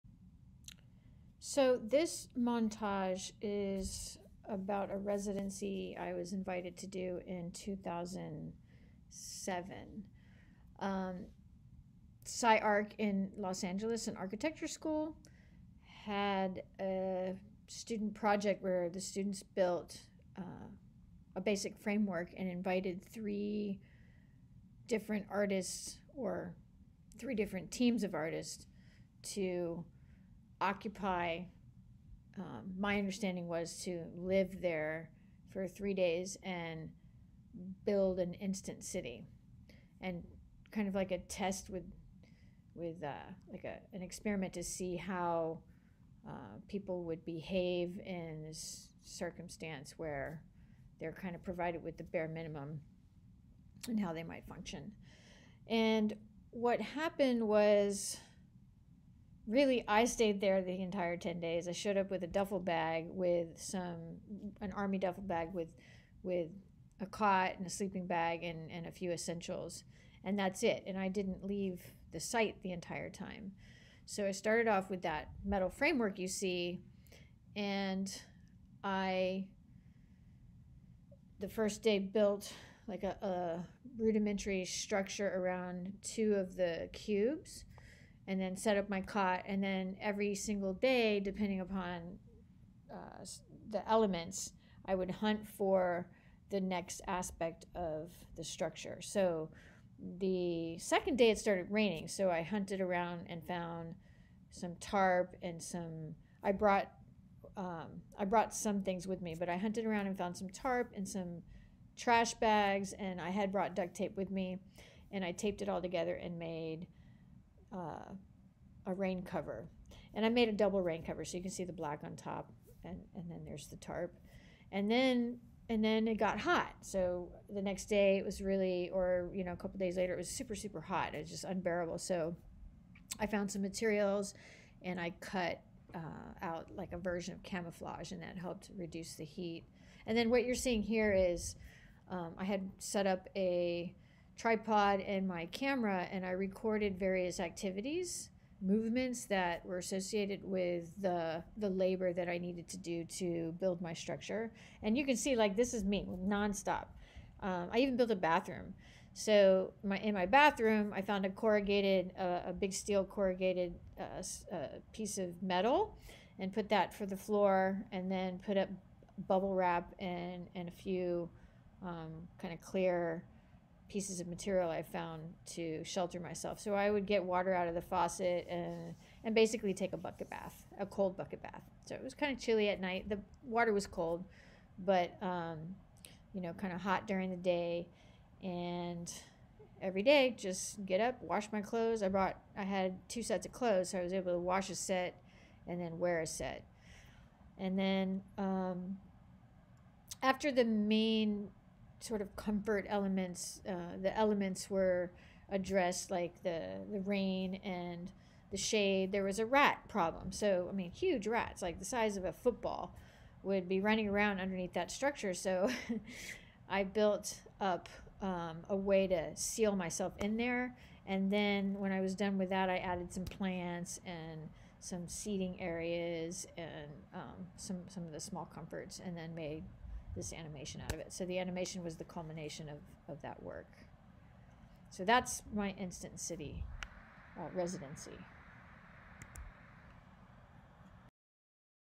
2007/18 Instant City with Voice Over
This montage is a compilation of documentation I made during a residency at SCI-Arc in Los Angeles.